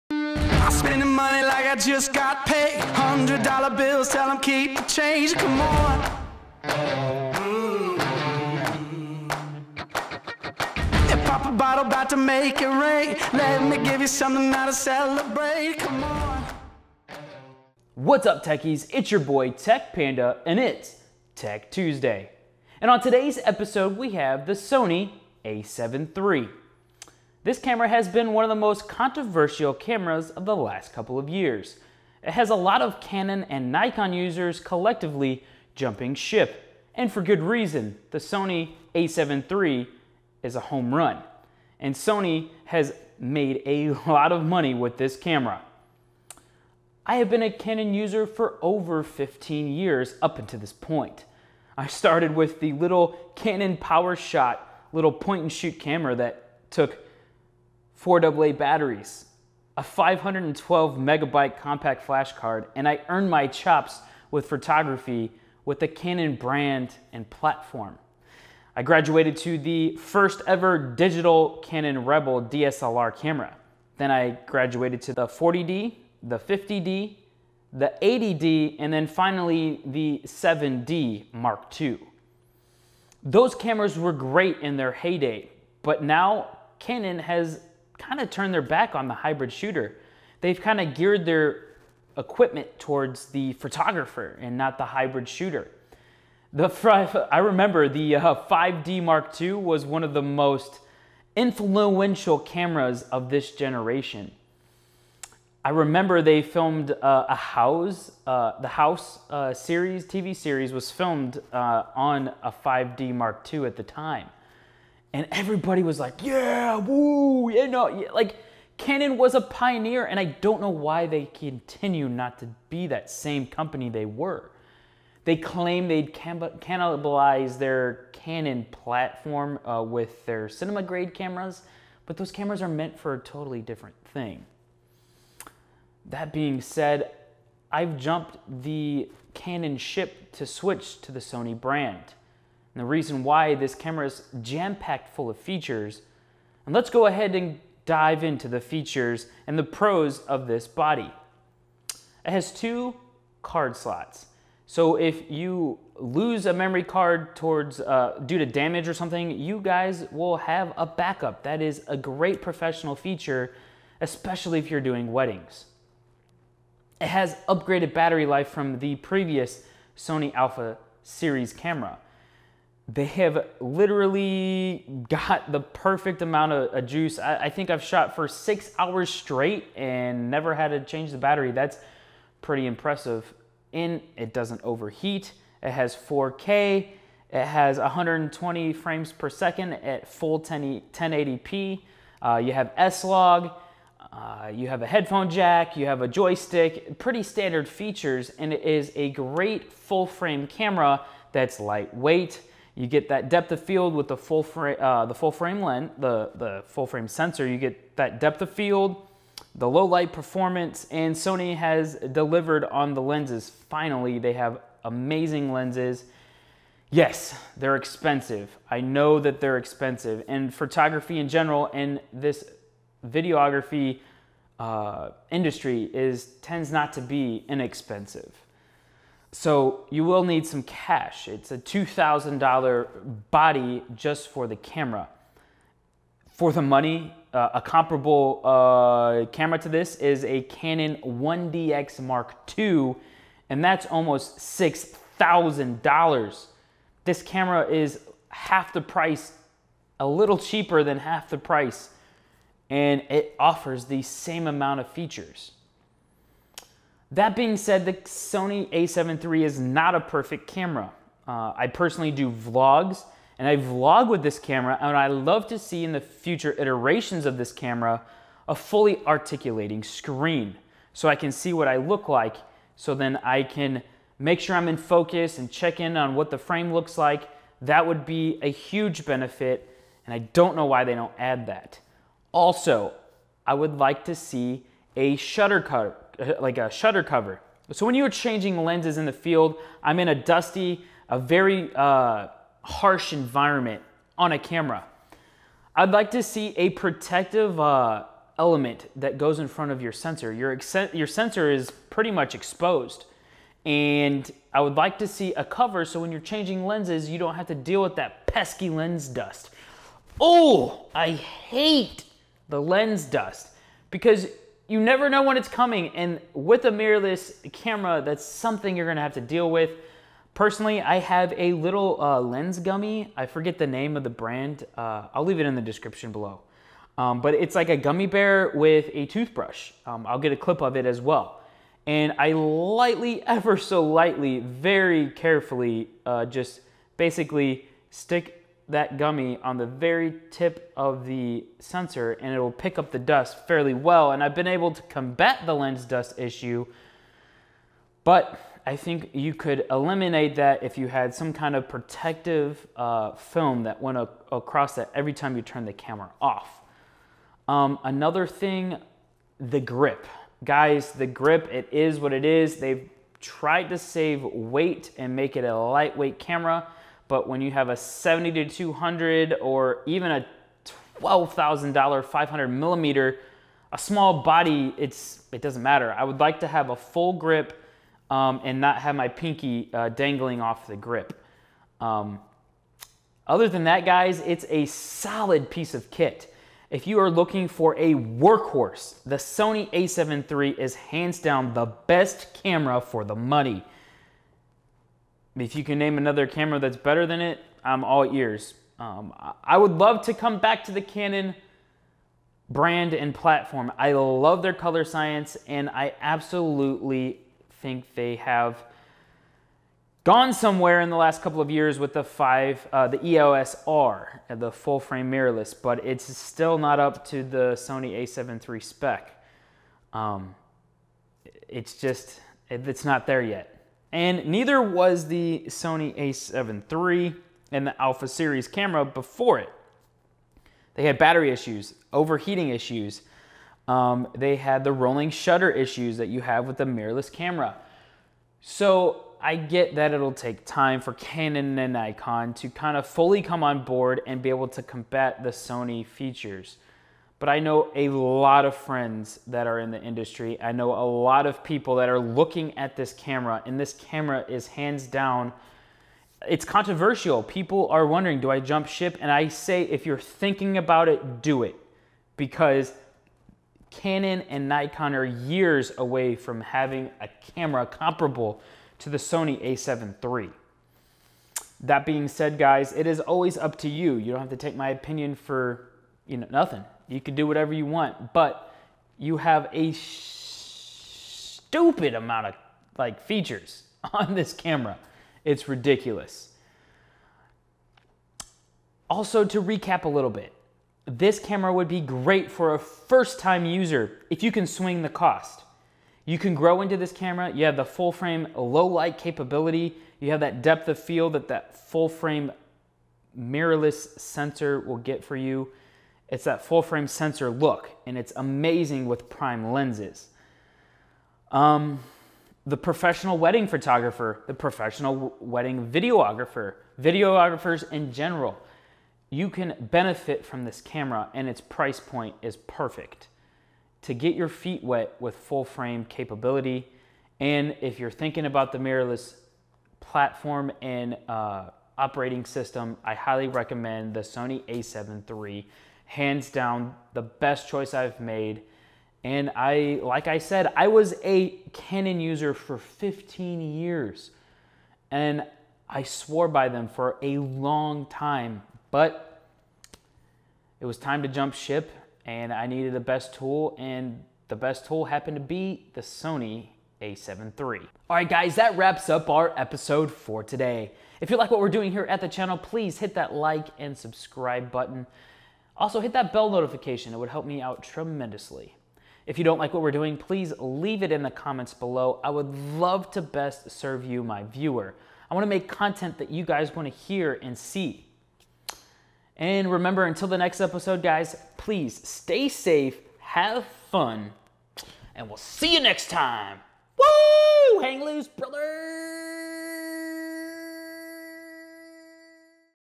SONY A7III REVIEW || CANON & NIKON VS SONY || TEKH TUESDAY
It's Tekh Tuesday! As a 15-year CANON user, I made the official switch to SONY. Watch as we over the pros and cons of this camera body, who this camera is meant for and whether or not you should make the SONY switch!

LENSES:
12-24MM: